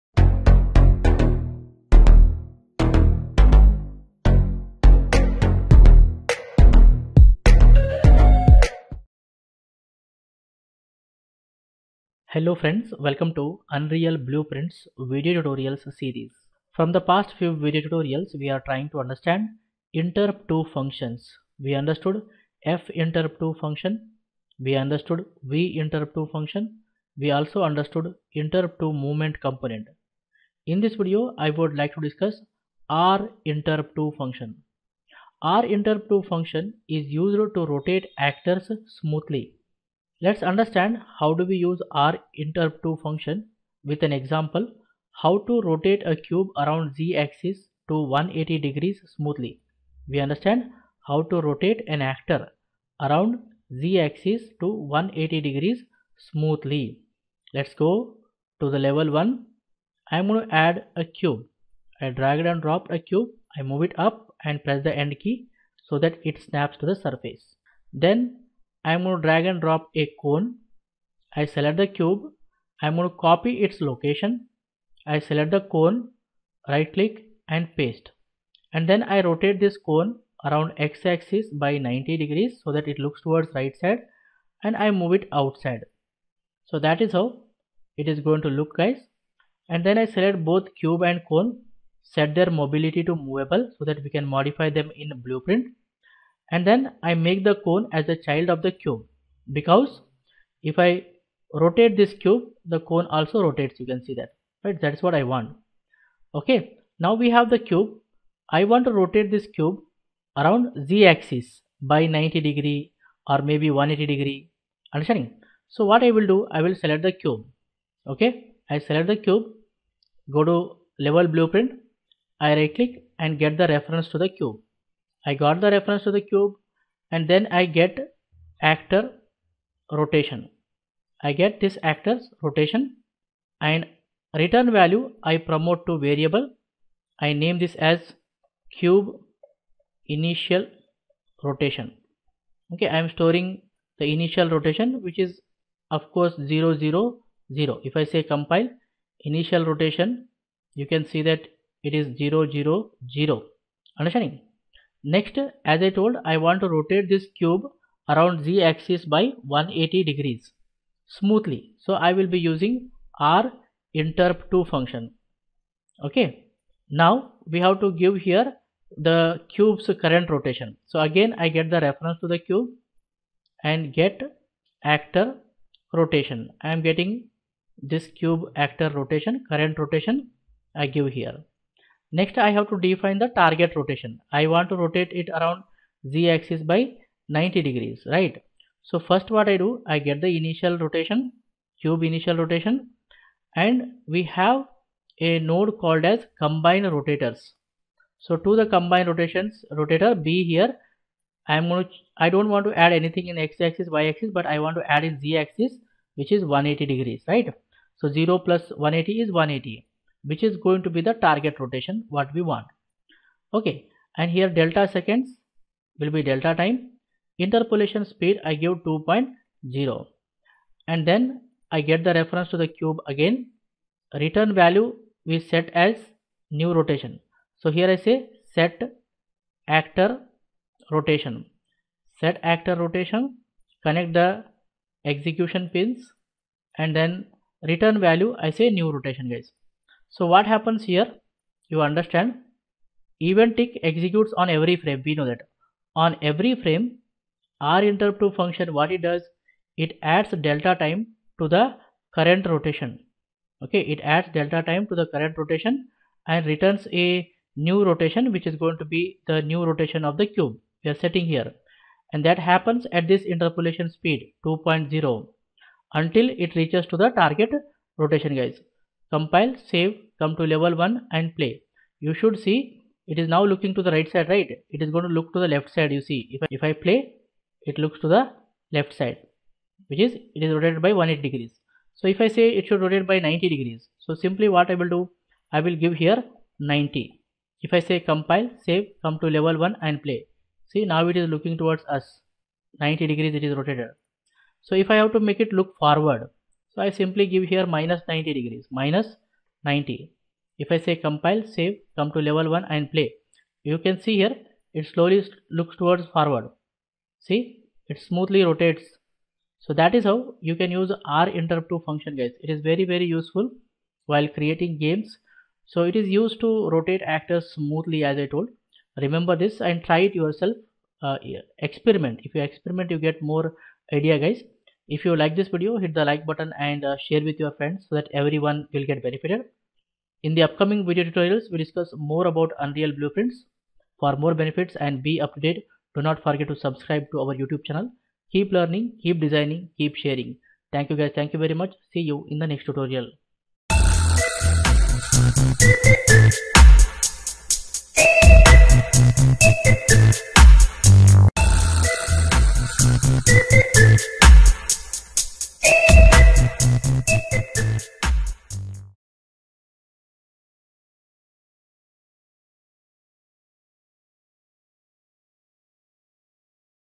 Unreal Blueprint RInterpTo Node - Unreal Blueprint Tutorial 31 🚀
► Master the RInterpTo node in Unreal Blueprint. This tutorial will guide you through creating smooth rotational transitions for your game objects, enhancing your overall game development skills.